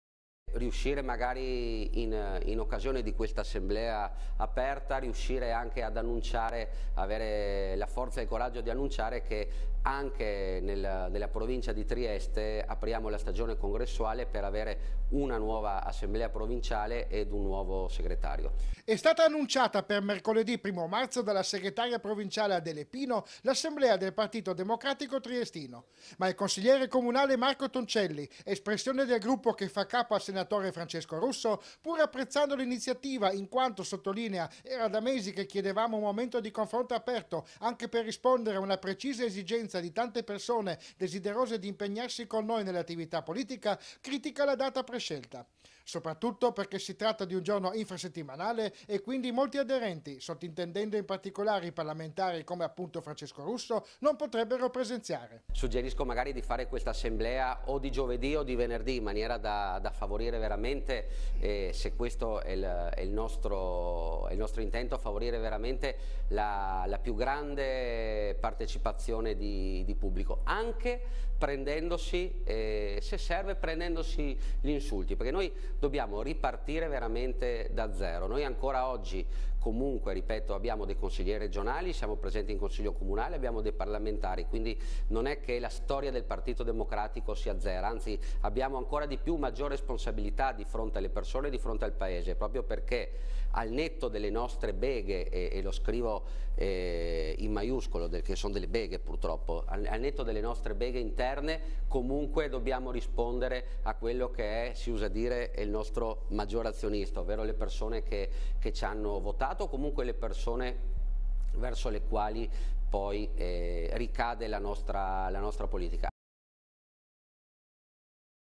21/02/2017 - TONCELLI (PD): LA DATA DELL'ASSEMBLEA ESCLUDE LA PRESENZA DEI PARLAMENTARI
IL NOTIZIARIO (martedì 21 febbraio 2017) - E’ stata annunciata per mercoledì 1 marzo dalla segretaria provinciale Adele Pino l'assemblea del partito democratico triestino. Ma il consigliere comunale Marco Toncelli, espressione del gruppo che fa capo al sen. Francesco Russo, pur apprezzando l'iniziativa in quanto, sottolinea, era da mesi che chiedevamo un momento di confronto aperto anche per rispondere a una precisa esigenza di tante persone desiderose di impegnarsi con noi nell'attività politica, critica la data prescelta. Soprattutto perché si tratta di un giorno infrasettimanale e quindi  molti aderenti, sottintendendo in particolare i parlamentari come appunto Francesco Russo, non potrebbero presenziare.